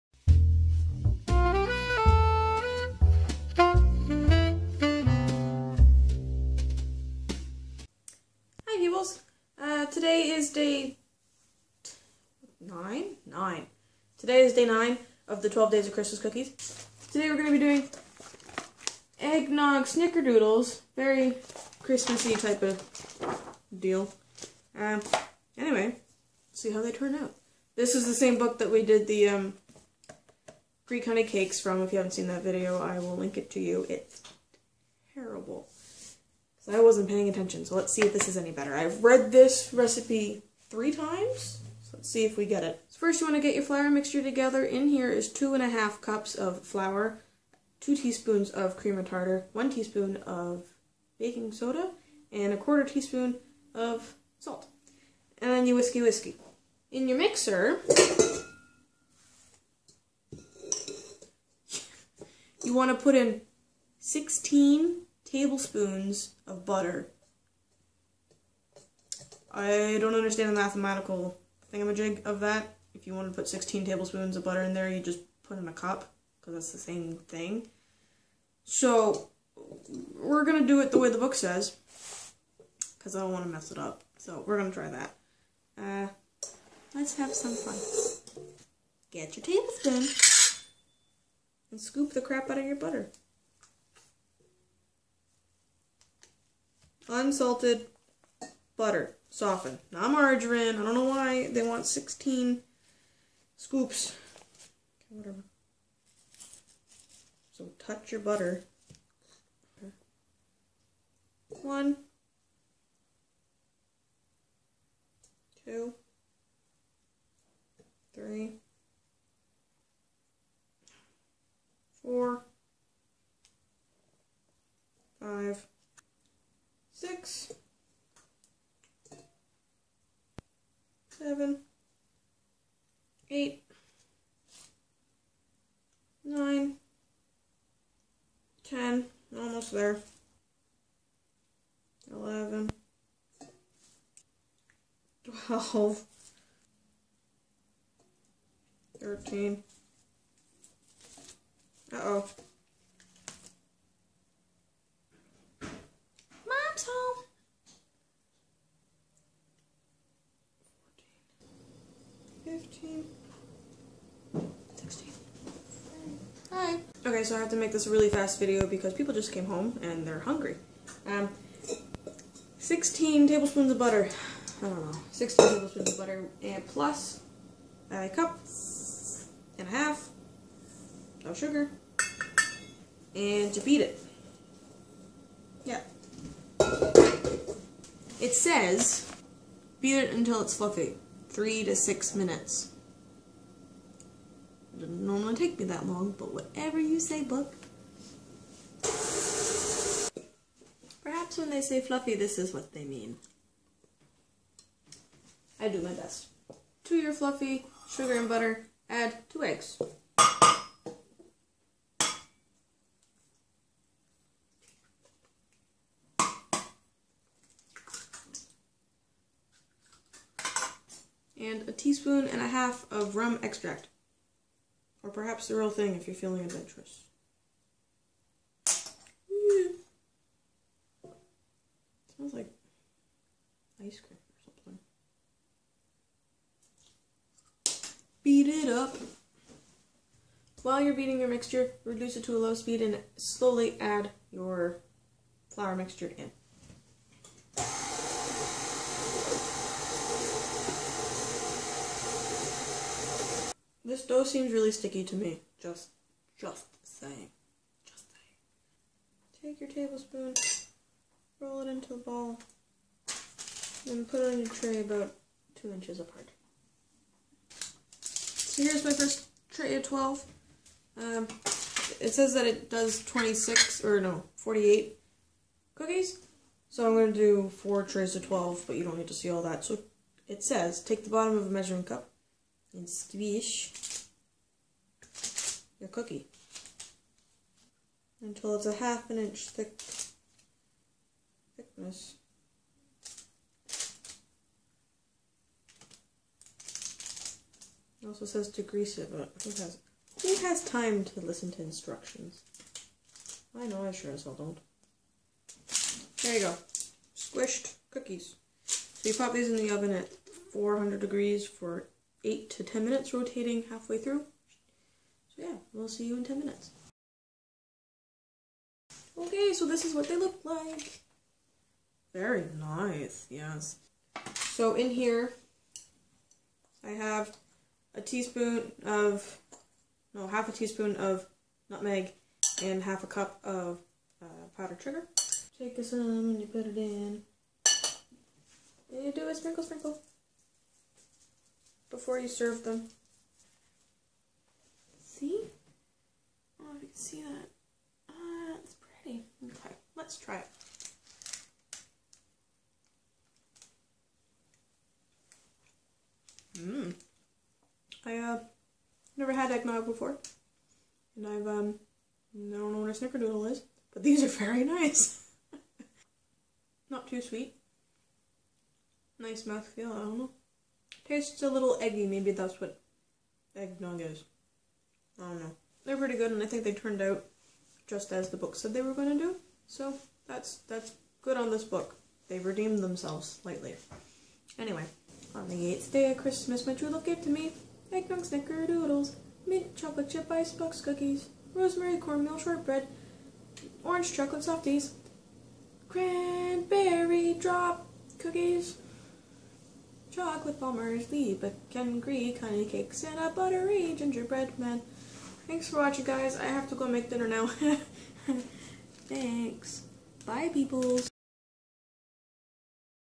The 12 Days of Christmas Cookies - Day 9
The 12 Days of Christmas Cookies!
Day 9!

On the 9th day of Christmas, my true love baked for me:
Eggnog Snickerdoodles
Mint Chocolate Chip Ice-box Cookies
Rosemary-Cornmeal Shortbread
Orange Chocolate Softies
Cranberry Drop Cookies!
Chocolate Palmiers
Lebkuchen
Greek Honey Cakes
And Buttery Gingerbread Men!

This recipe made a lot (A LOT) of dough. So if you would like to make some at home, but don't want 48 (or more) cookies, just half the recipe. Make sure you sprinkle them with the sugar mixture before serving them to guests or family, 'cause they will looks just that much more elegant.

These actually turned out really nicely, and really tasty. I would make them again.

Does anyone know what an snickerdoodle is? :/

- Love Dana and Tegan